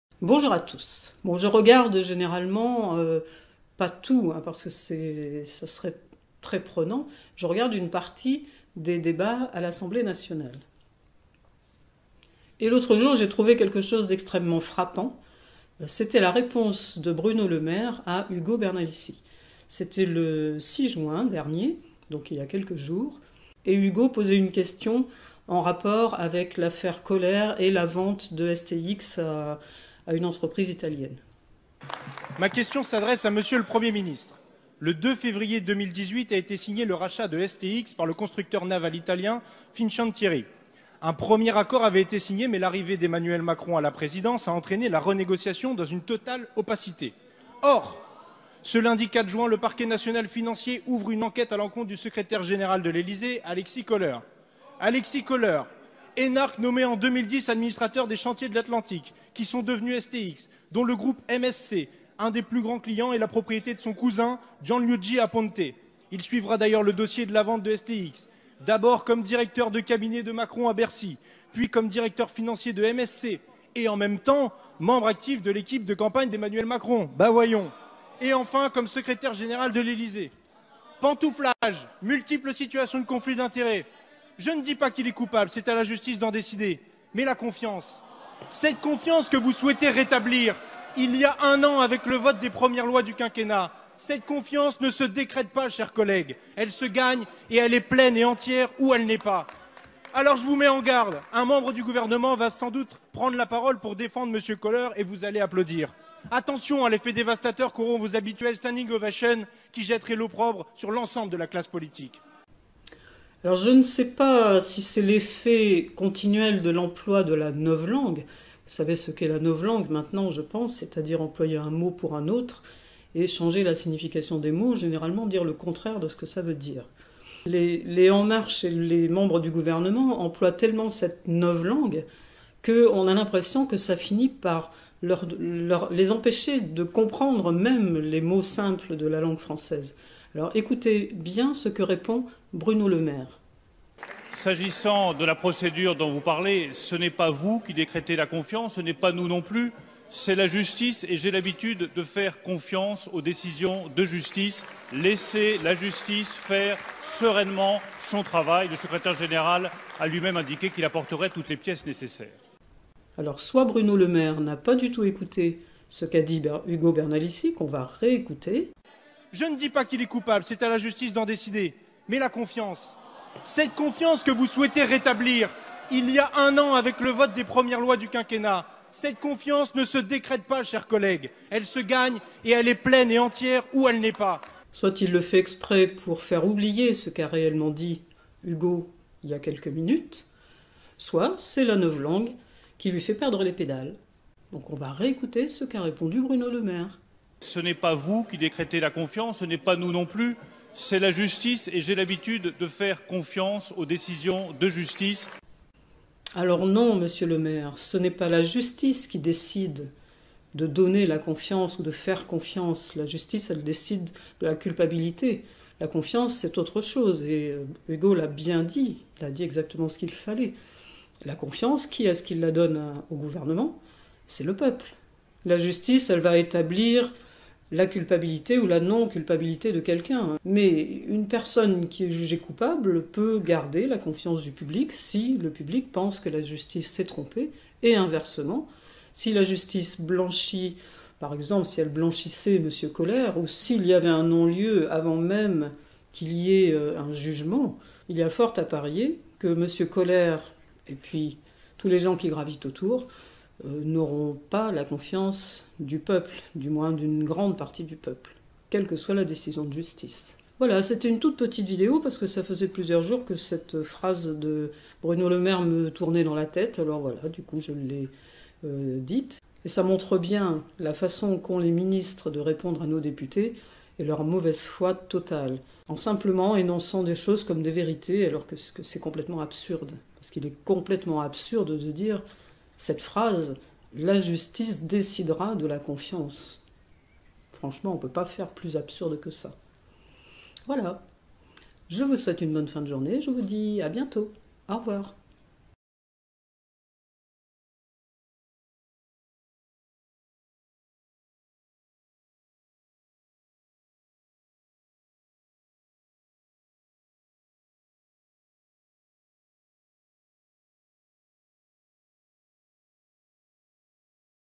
LREM : L'ABUS DE LA NOVLANGUE LEUR FAIT PERDRE LEUR FRANÇAIS...
Quand Ugo Bernalicis pose une question et que Bruno Le Maire lui répond en ne sachant plus ce que parler français veut dire...

Ma page Facebook. Ma page insoumise